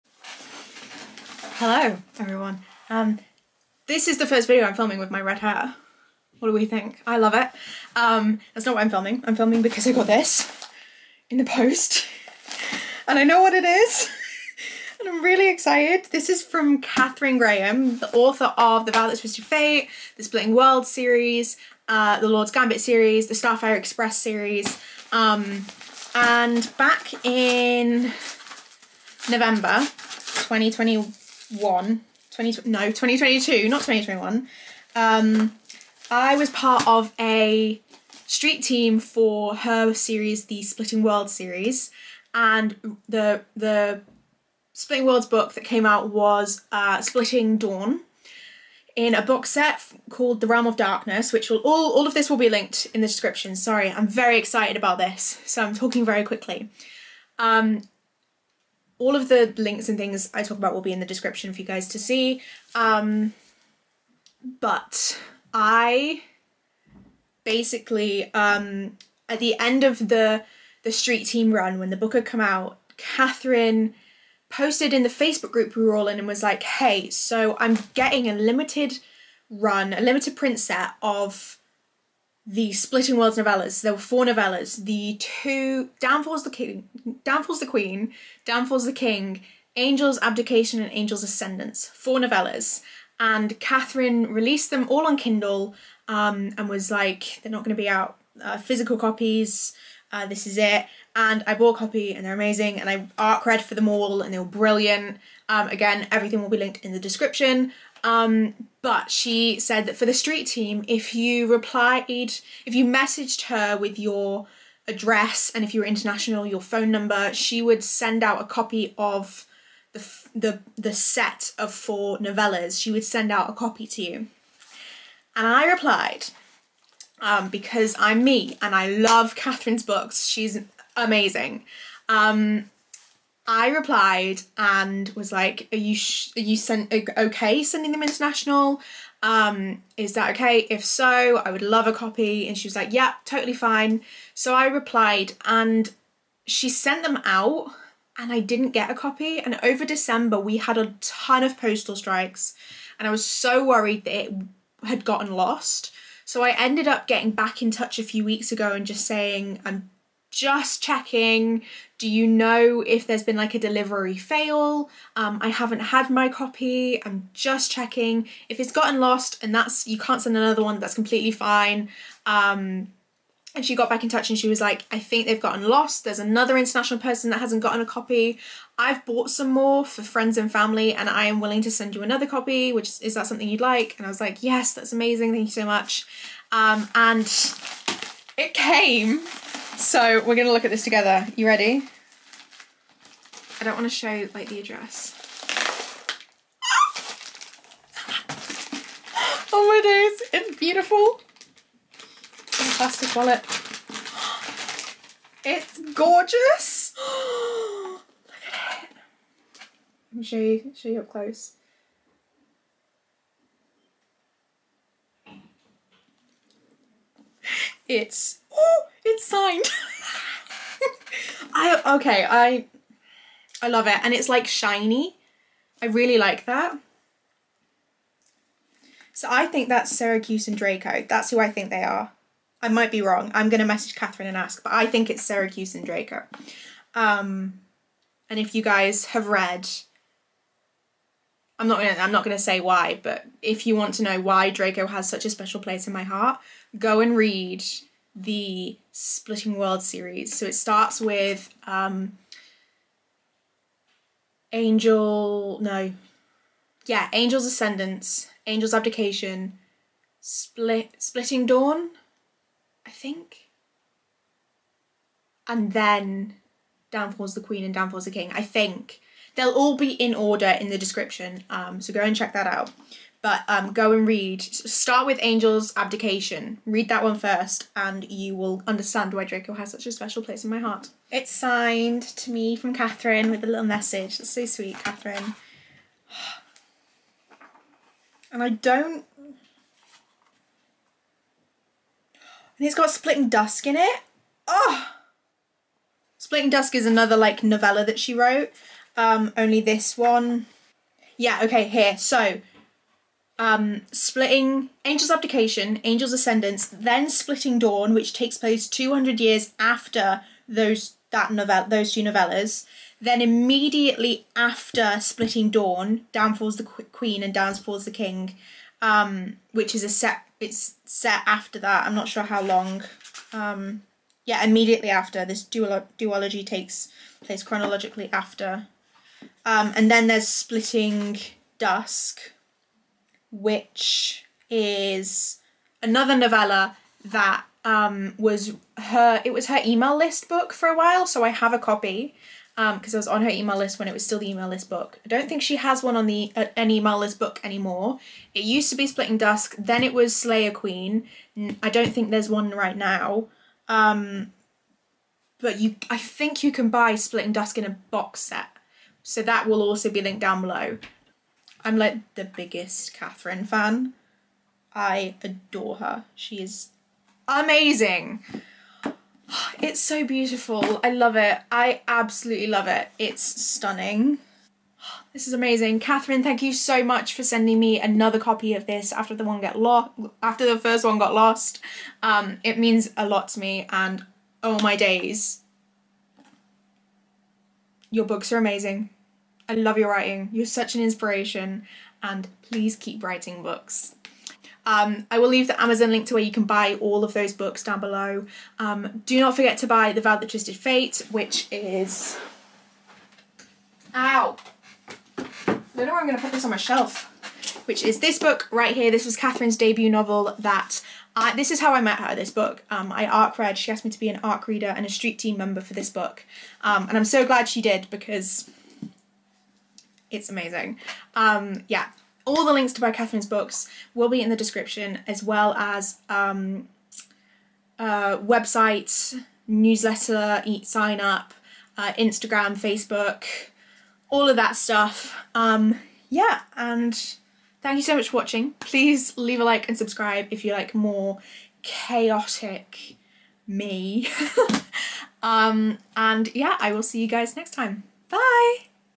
Marvellous March: Splitting Worlds Unboxing
I got bookish post!!!I’ve been waiting for this since last year and I am so excited about it!

Thank you so much to Katherine for shipping me not one, but two copies (after the first copy got lost in transit)

The Lords Gambit Series

The Vow That Twisted Fate

Splitting World Series

Splitting Dawn

Slayer Queen